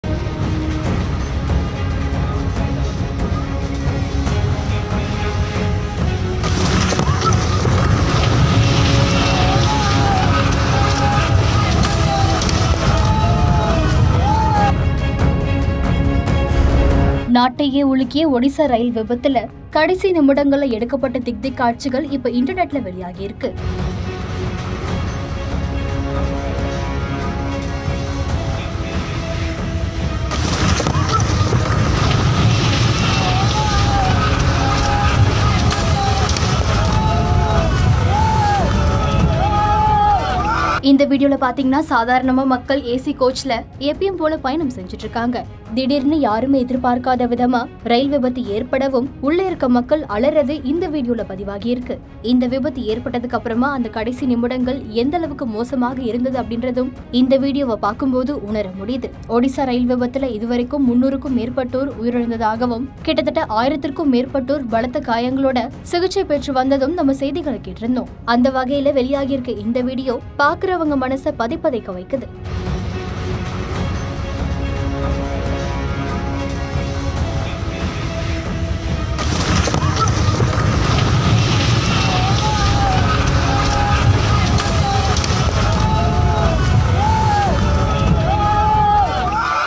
நிம்மதியாக உறங்கிக்கொண்டிருந்த மக்கள்.. Coromandel Express-ன் கடைசி நிமிடங்கள்.!
Coromandel Express-ன் கடைசி நிமிடம்.. பரபரப்பான Video காட்சி!